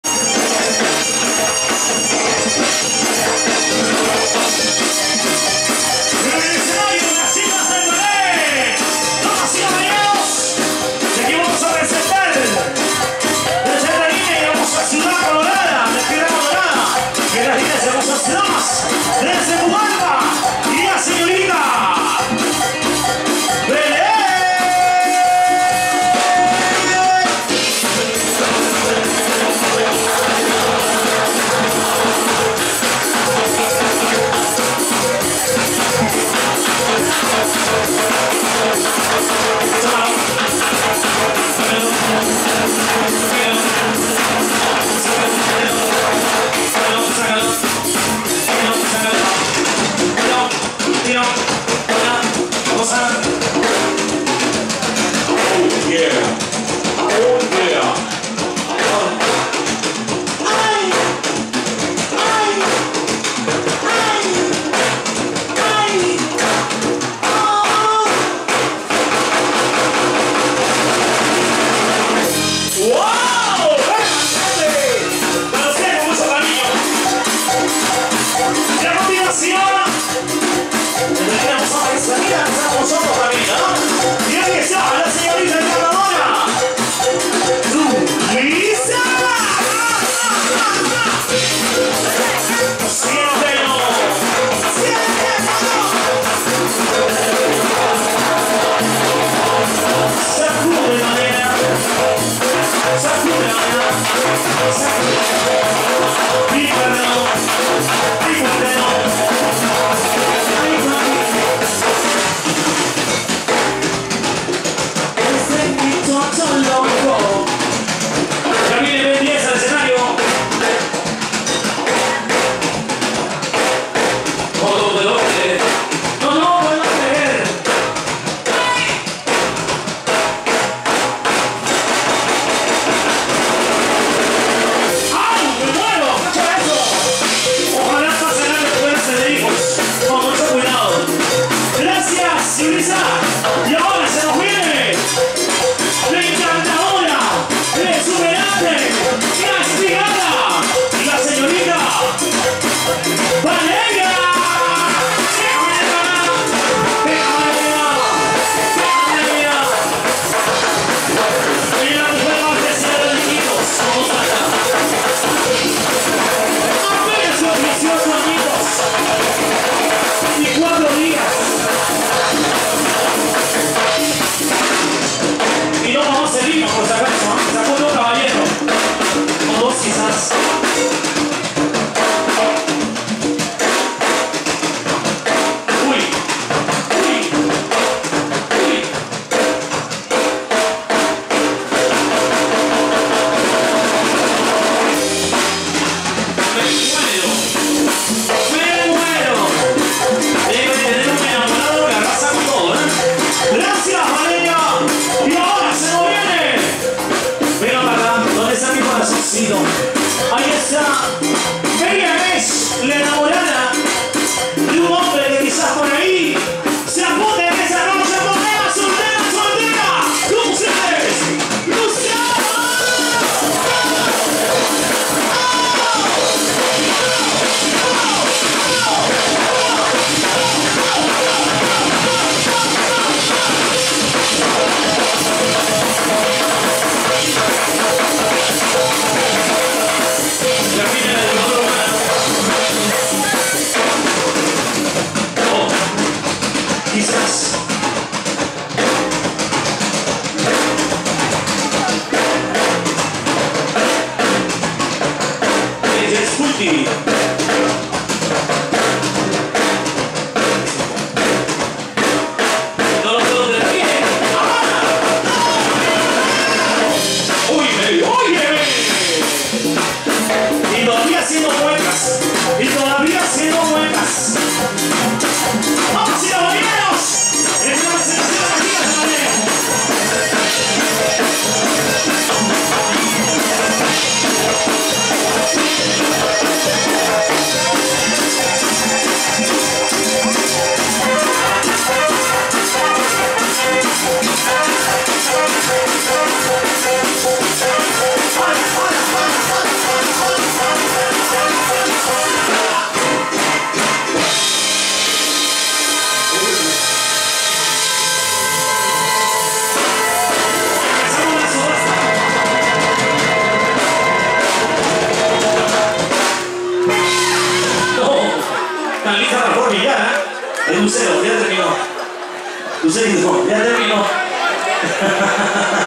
BAILARINAS DE EXPLOSION DE IQUITOS EN LA ISLA DEL PARAISO EN HALLOWEEN 31/10/2011
PRESENTACION DE BAILARINAS DE EXPLOSION DE IQUITOS,  QUE SE OFRECIO EN LA FIESTA POR HALLOWEEN EN DIA 31 DE OCTUBRE DE 2011 EN LA ISLA DEL PARAISO EN LIMA,  PRESENTARON A: DOS NUEVAS BAILARINAS: A LAS LINDAS PUCALLPINAS BELEN, Y JULISSA Y A LAS BELLAS VALERIA Y LUCERO NATURALES DE IQUITOS, FUE UNA BUENA PRESENTACION